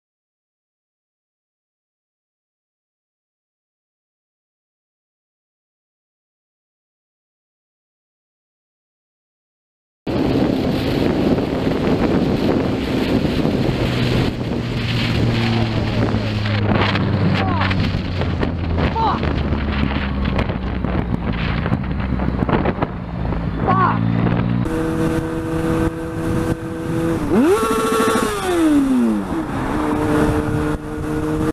Merging Monday Week 3 - PREXY / Salisbury Hwy
Merging Monday Week 3 - 20/02/17

Port River Expressway / Salisbury Hwy at Wingfield where the South Rd Superway (M2) ends!

Very close shave for the motorcyclist on this horrible merge, always check your blind spots before merging as this almost ended in an accident!